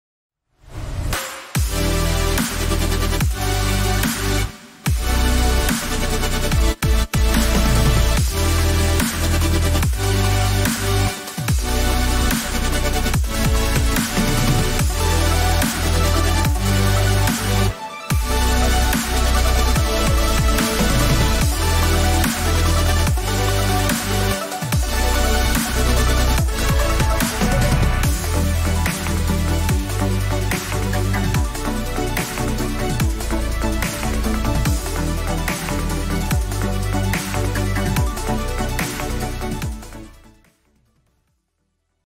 Trailer - App Epargne Salariale
Une nouvelle application, plus simple, plus claire et plus direct pour gérer votre épargne salariale.